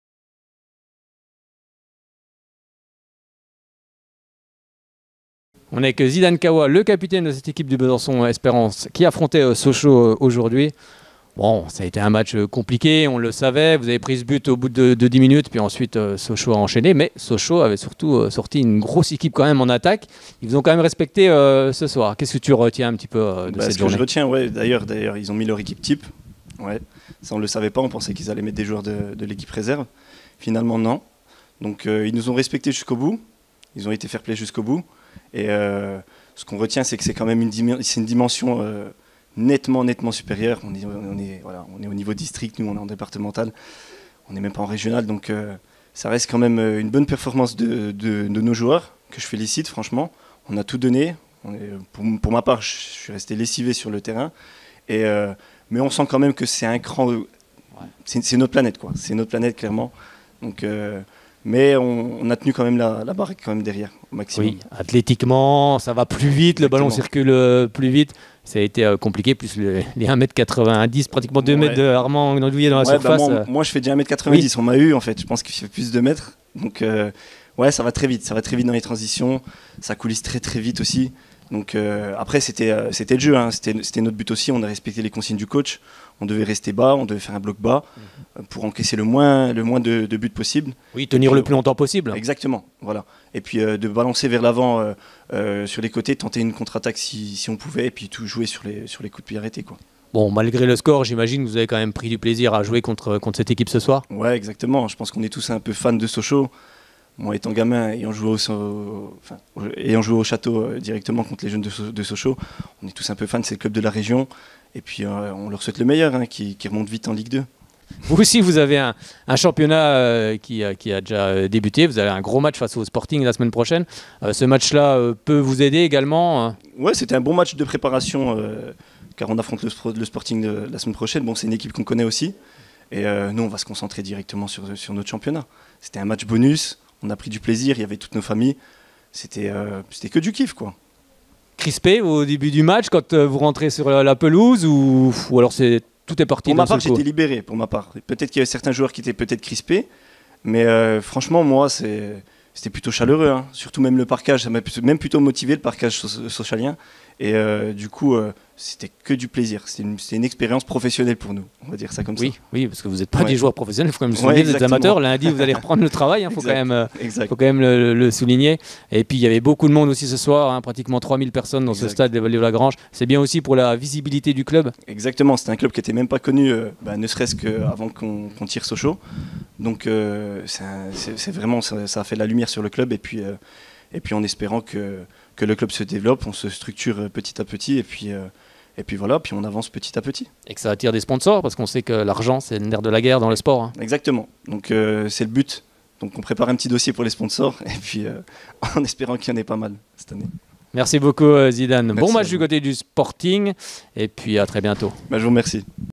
Zidane Khaoua (AS Besançon Espérance / Sochaux) : " C'était que du kif "
Retrouvez la réaction du capitaine de l'AS Besançon Espérance Zidane Khaoua au micro de Frédéric Chevassut (Soyons Sport) après l'élimination au 5ème tour de la Coupe de France face à Sochaux (0/6).

Passez nous voir sur notre page facebook
Soyons Sport
et sur twitter
Nouveau aussi le site internet Soyons Sport